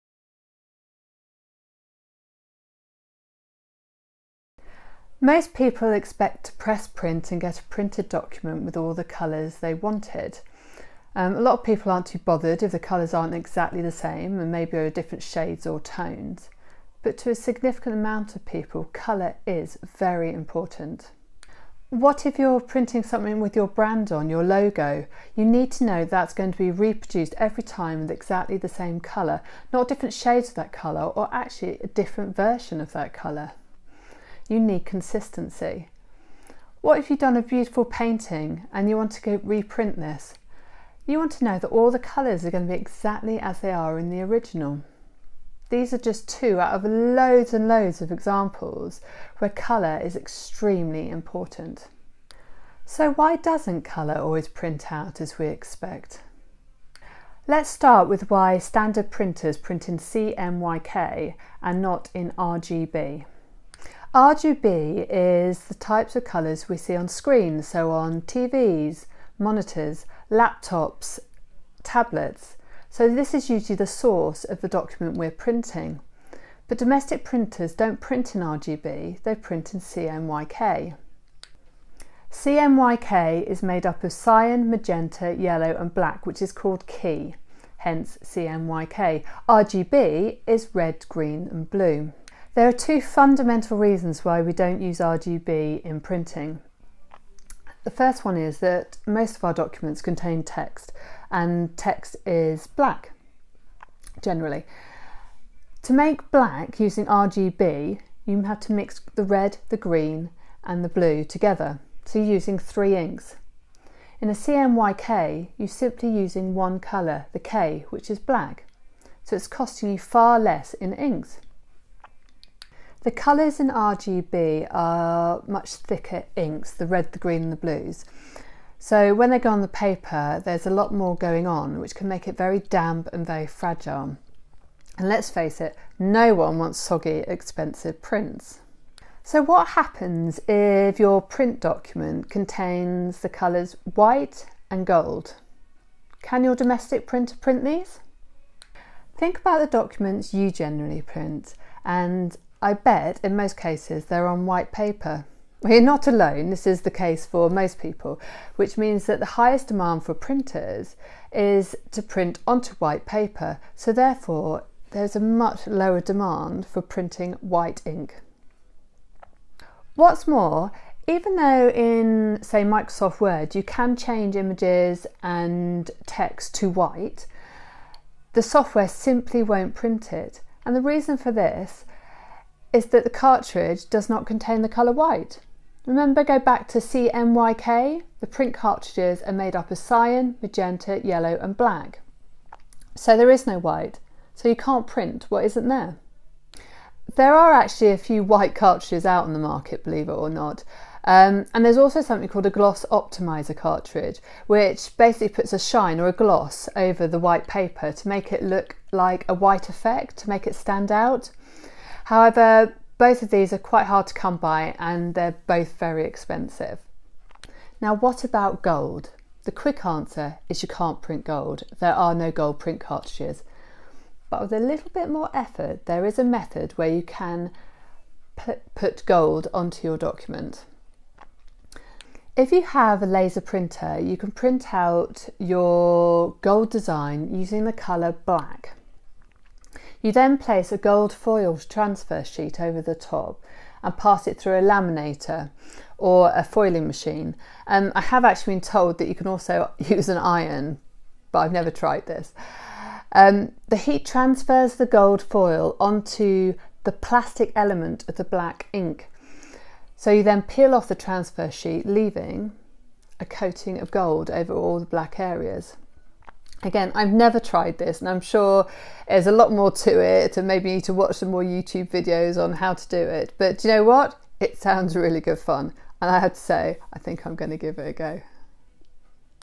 What is White and Gold All Over? (Apparently not your printer document!)
Most people when they have a document they want to print, just press print and assume they’ll get a finished print-out containing all the colours in their original (onscreen) document. A lot of people aren’t too bothered about whether the colours are exactly the same, or different tones and shades, but to a significant number of people, colour is extremely important.
So why doesn’t a colour always print out as we expect?